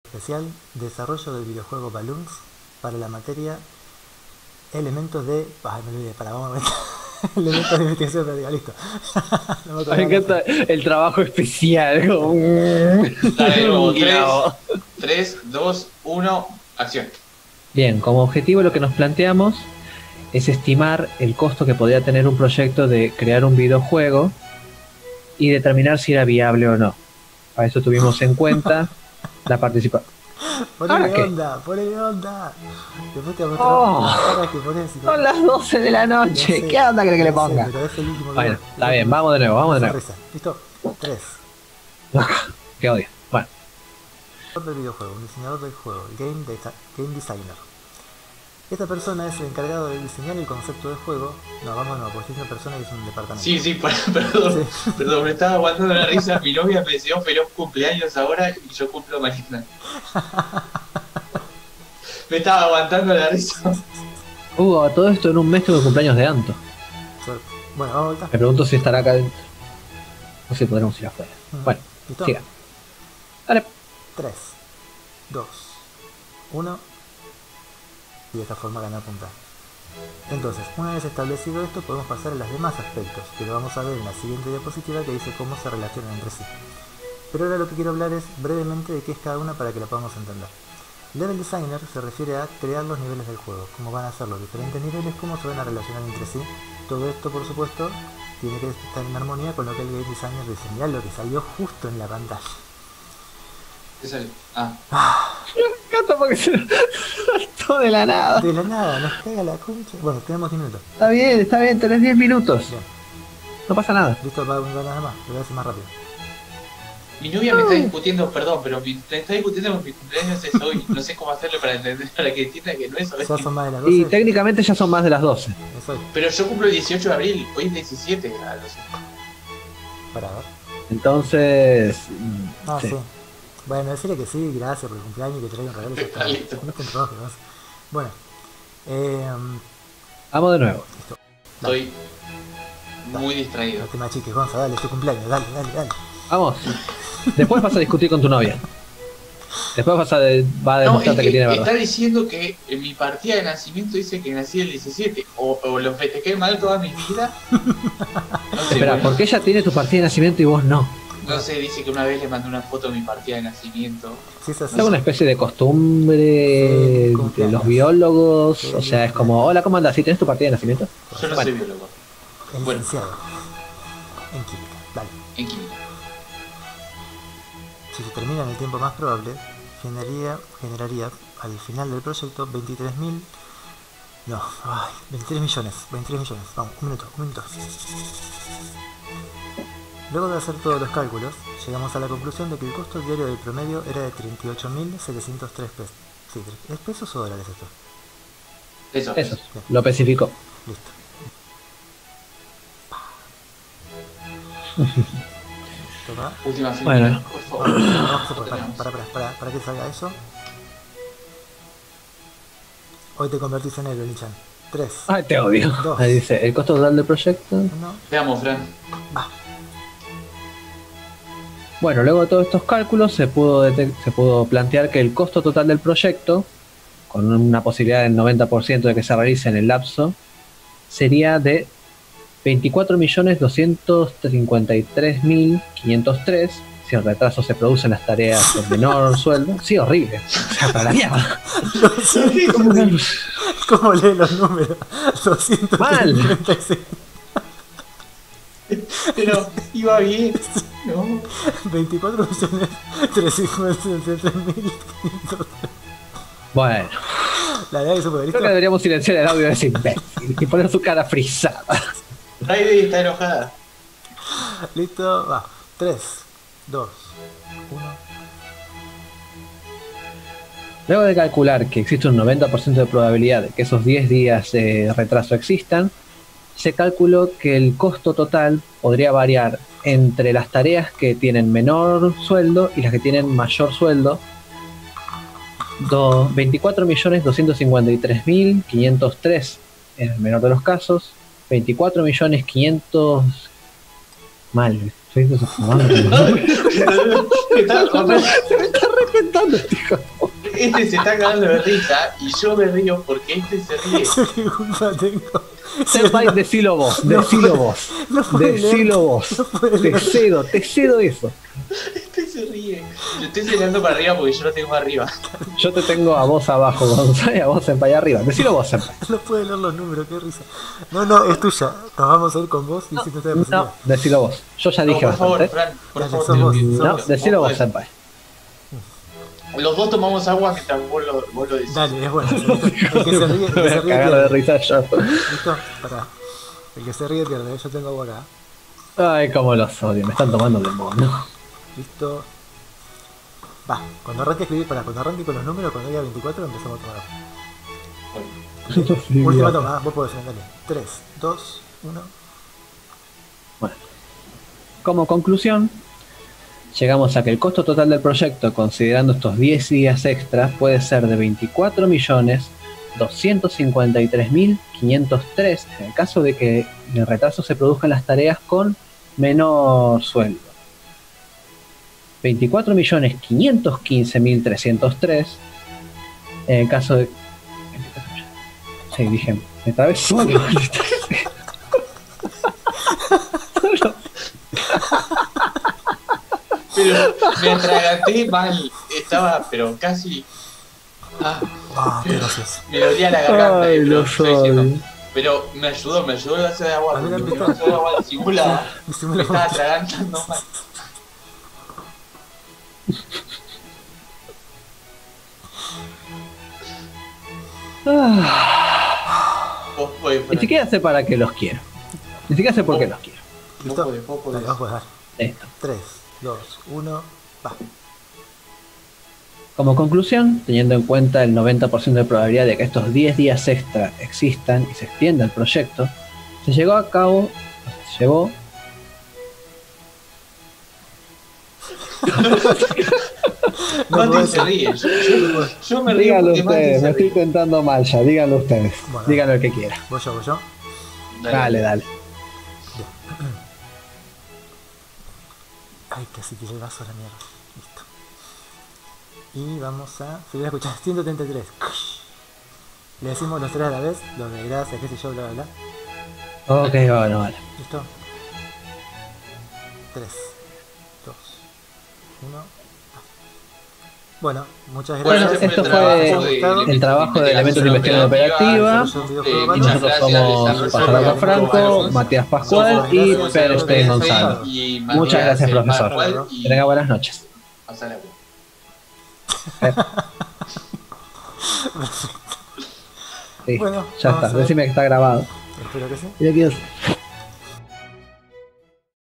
Todo lo que nos salio mal en la presentacion de Investigacion Operativa
Un compilado con todas las cosas en las que nos equivocamos